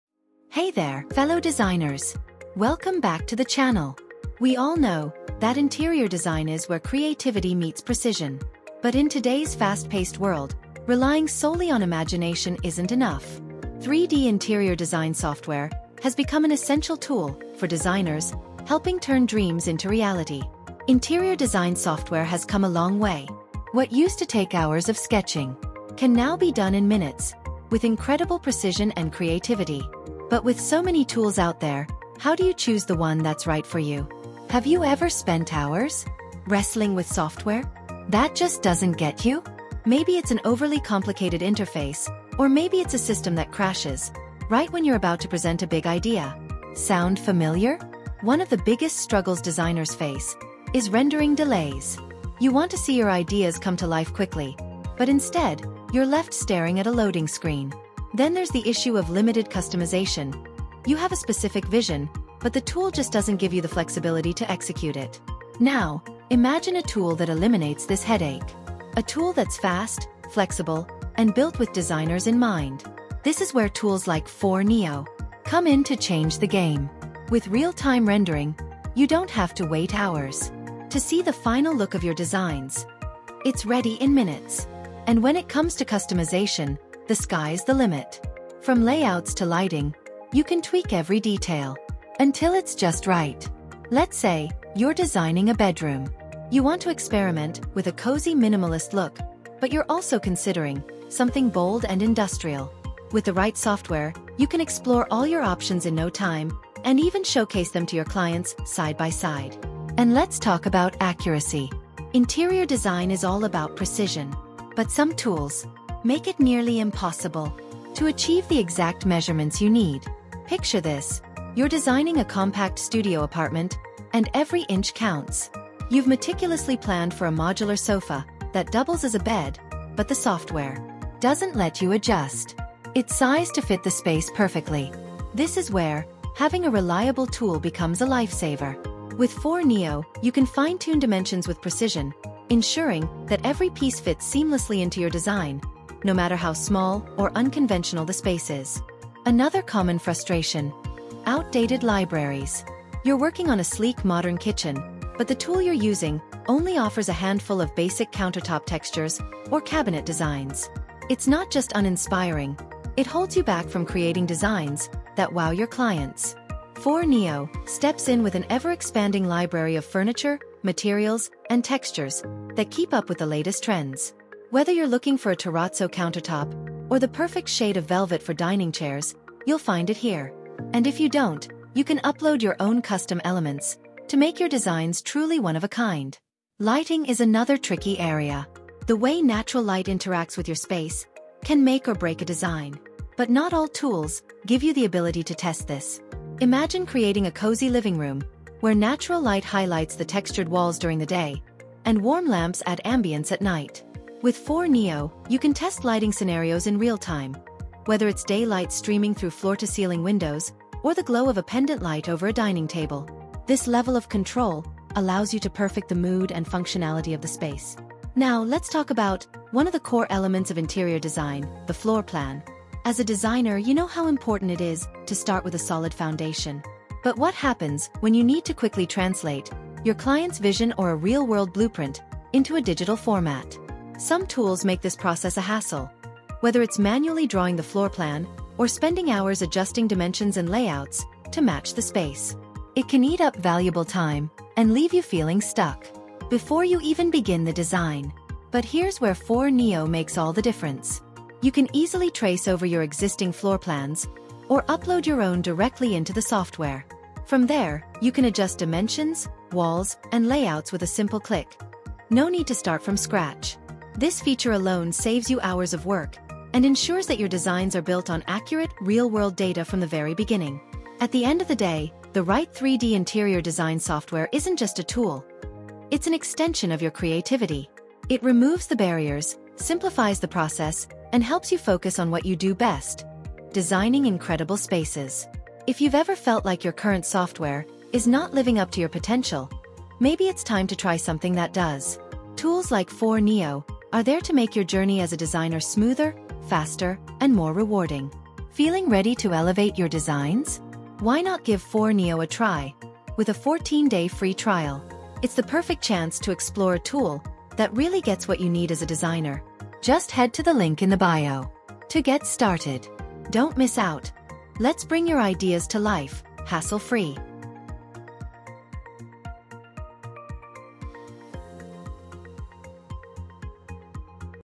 The Best 3D Interior Design Software You Need in 2025!
Discover why Foyr Neo is the top choice for 3D interior design software in 2025 with our latest video! Learn about its user-friendly interface, powerful features like real-time rendering and cloud-based access, and see how it stands out against competitors.

Watch real-world examples of stunning designs created with Foyr Neo, and understand why it’s the tool you need to stay ahead in the fast-evolving design industry.

Timeline of the video-

0:00 - 0:47 - Introduction
0:48 - 1:05 - Biggest struggles designers face
1:06 - 1:50 - Solution
1:51 - 2:15 - Accuracy in Designs
2:16 - 2:28 - Solution
2:29 - 2:46 - Outdated libraries
2:47 - 3:06 - Solution
3:07 - 3:23 - Lighting is another tricky area.
3:24 - 3:37 - Solution
3:38 - 4:08 - The Floor Plan
4:09 - 4:31 - Solution
4:32 - 5:25 - Ending